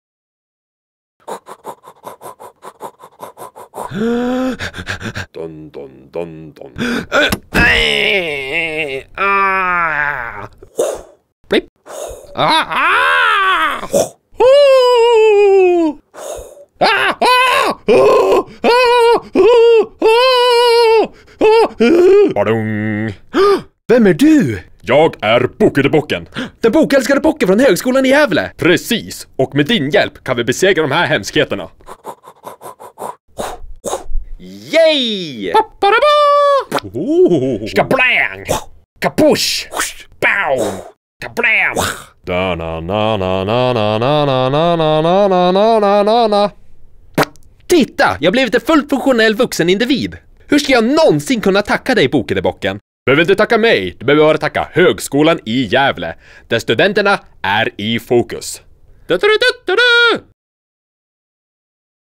Bokedebocken
Det är enkelt att känna ångest över vuxenlivet och inte veta vad man ska göra när det börjar. Ibland kanske man bokstavligen blir attackerad av vuxenlivet självt…
Av: Mikael Sundberg